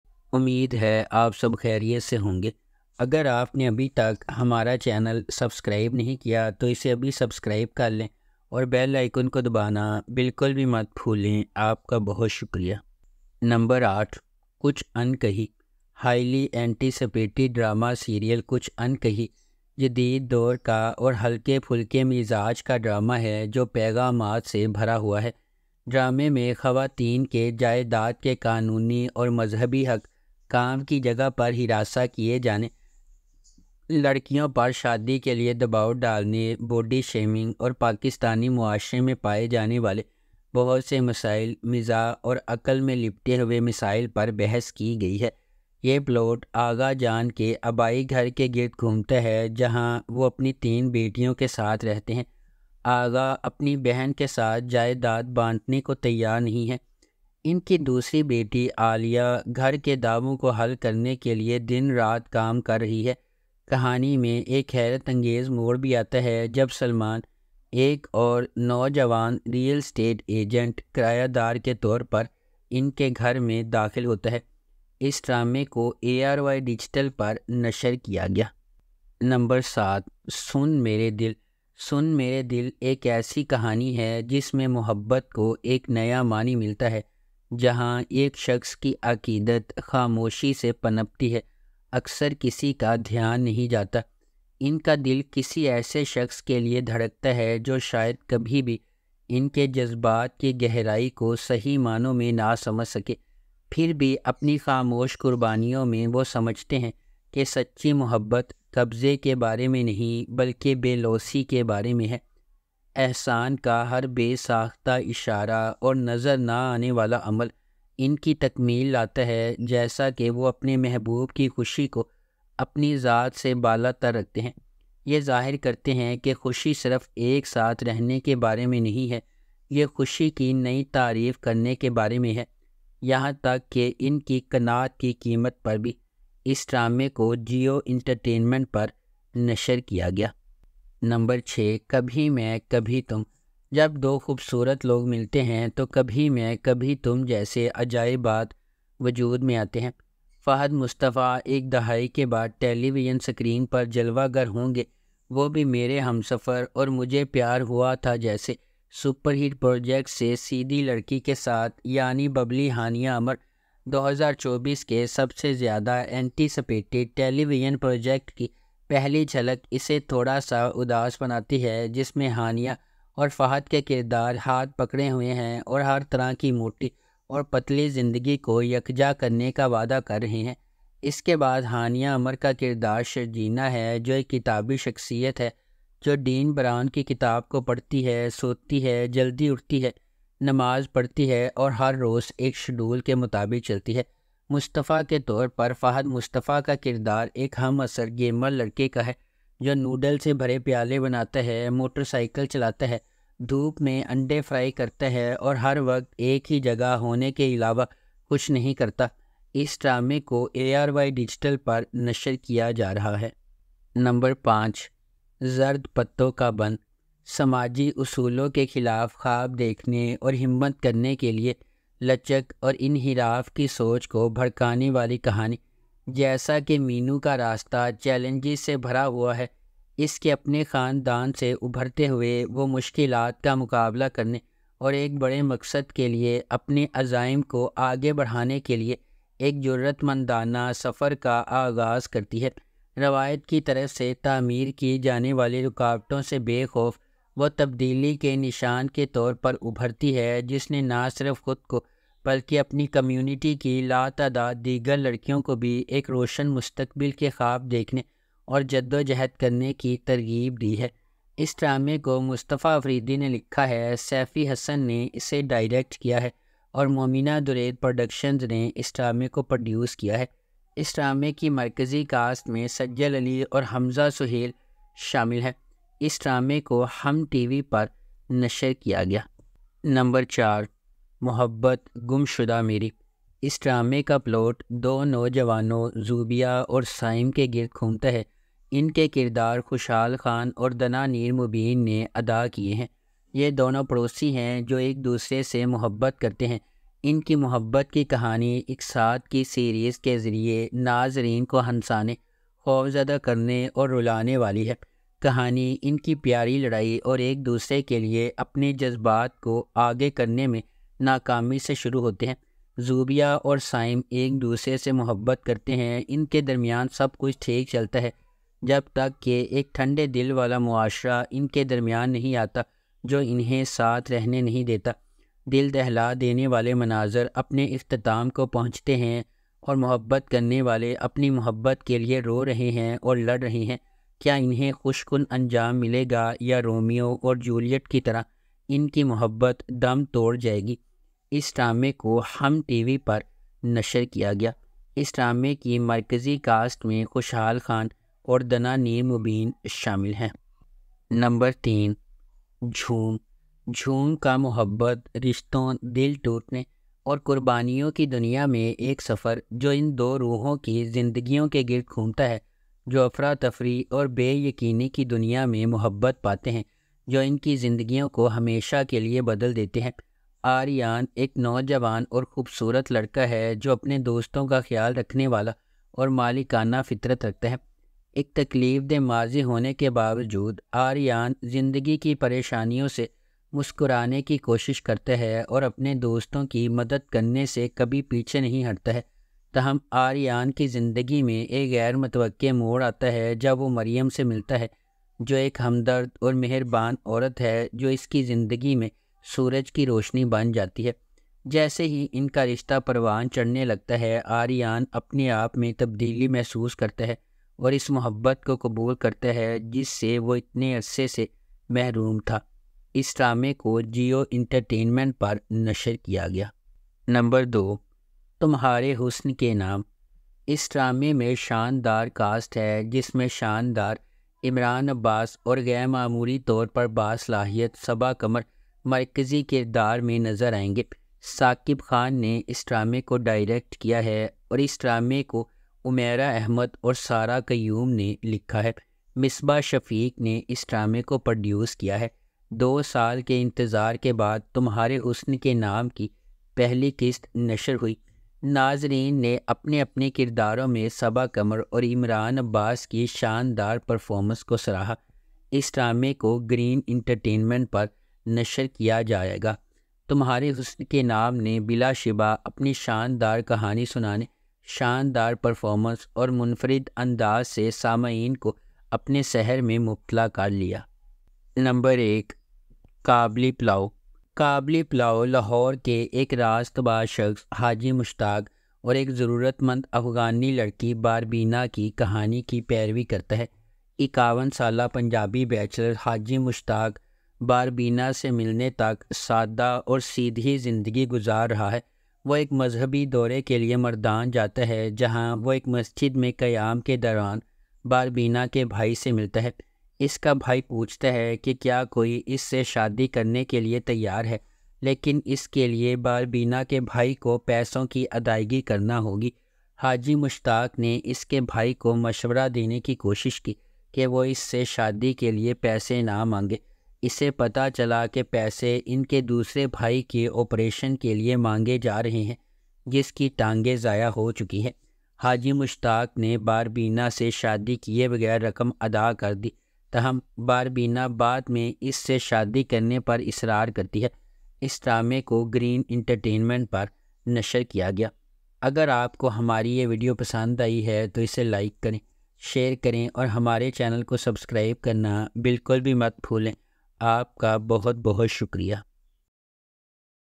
Top 8 Most Loved Pakistani Dramas With Beautiful Storyline
Pakistani Dramas Present Top 8 Most Loved Pakistani Dramas With Beautiful Storyline. This List Has Been Prepared After Our Details Research.  In This Video You Will See About Top 8 Most Loved Pakistani Dramas With Beautiful Storyline. We Tried Our Best to Perform It Accurately And We Hope You Like It.

Thanks.